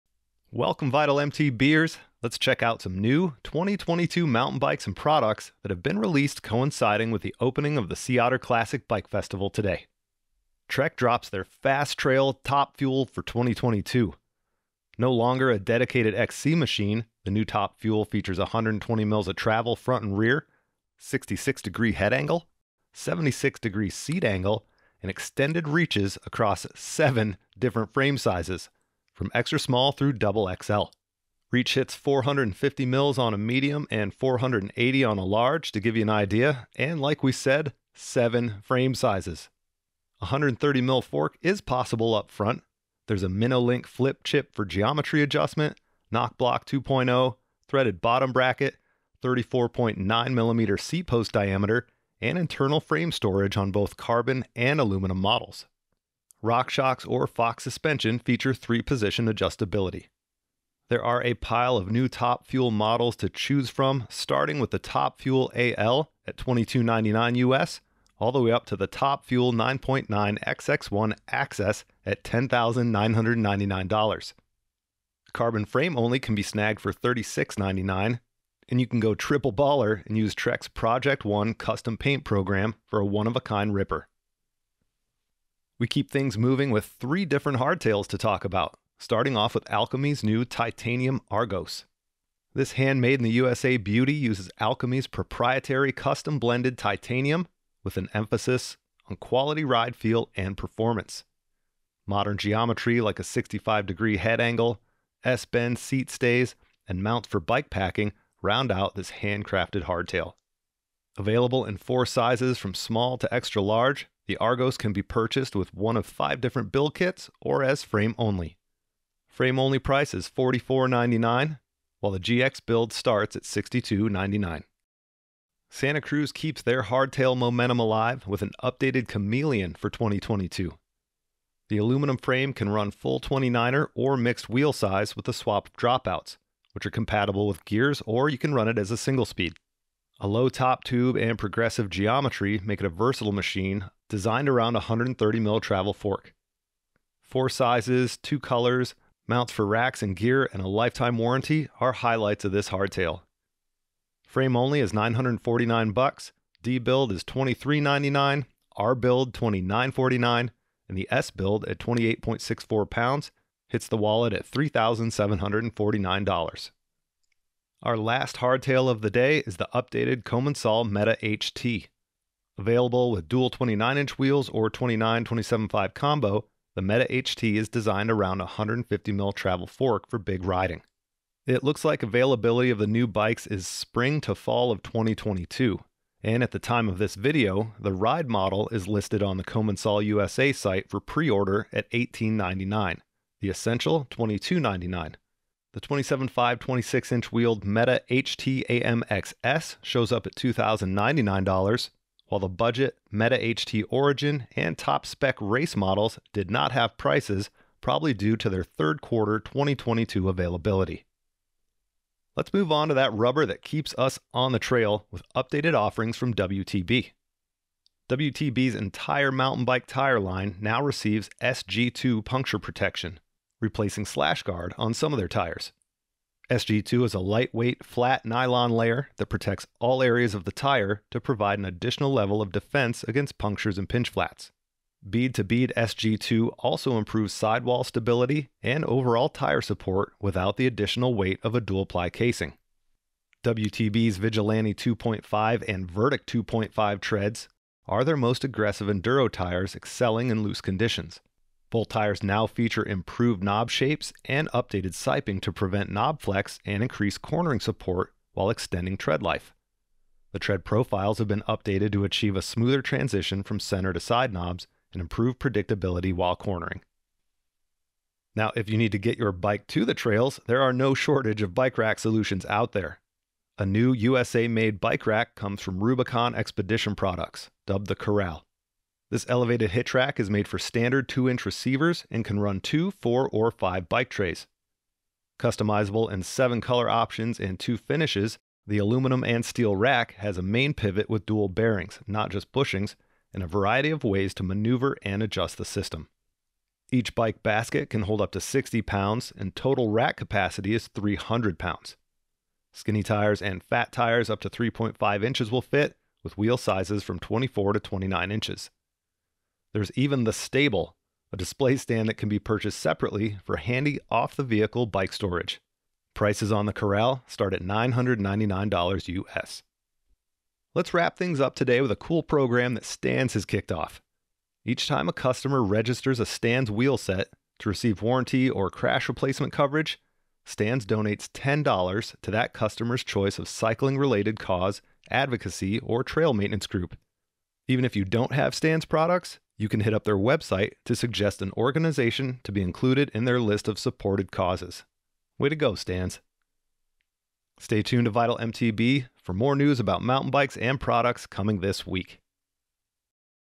4 New Mountain Bikes and 2022 Gear Updates
The 2021 Sea Otter Classic kicks off this morning. As one of the biggest cycling festivals in the country, new bikes, products and gear are shown off. We get a jump on some of the things that may be seen there.

Video Contents
0:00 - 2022 Trek Top Fuel
1:25 - Alchemy Argos Ti Hardtail
2:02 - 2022 Santa Cruz Chameleon
2:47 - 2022 Commencal Meta AM HT
3:31 - WTB Tire Updates
4:28 - RubiconEP Corral Rack
5:20 - Stan's Connection